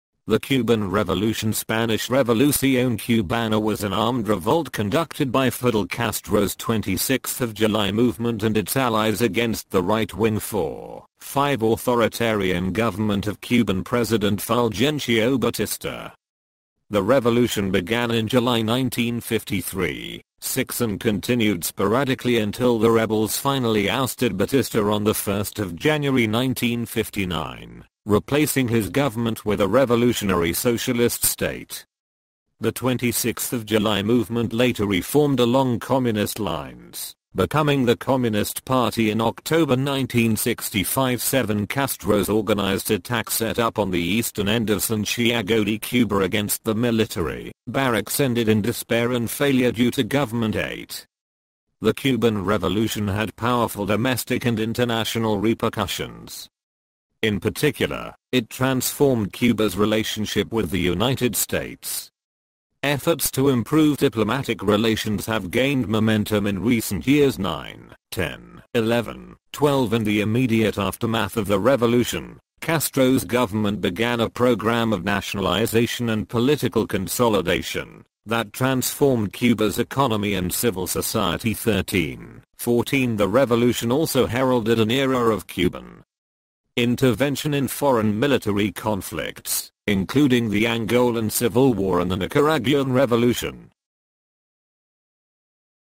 Cuban Revolution
The Cuban Revolution (Spanish: Revolución cubana) was an armed revolt conducted by Fidel Castro's 26th of July Movement and its allies against the right-wing[4][5] authoritarian government of Cuban President Fulgencio Batista. The revolution began in July 1953,[6] and continued sporadically until the rebels finally ousted Batista on 1 January 1959, replacing his government with a revolutionary socialist state. The 26th of July Movement later reformed along communist lines, becoming the Communist Party in October 1965.[7] Castro’s organized attack set up on the eastern end of Santiago de Cuba against the military barracks ended in despair and failure due to government.[8]

The Cuban Revolution had powerful domestic and international repercussions. In particular, it transformed Cuba's relationship with the United States. Efforts to improve diplomatic relations have gained momentum in recent years.[9][10][11][12] In the immediate aftermath of the revolution, Castro's government began a program of nationalization and political consolidation that transformed Cuba's economy and civil society.[13][14] The revolution also heralded an era of Cuban intervention in foreign military conflicts, including the Angolan Civil War and the Nicaraguan Revolution.[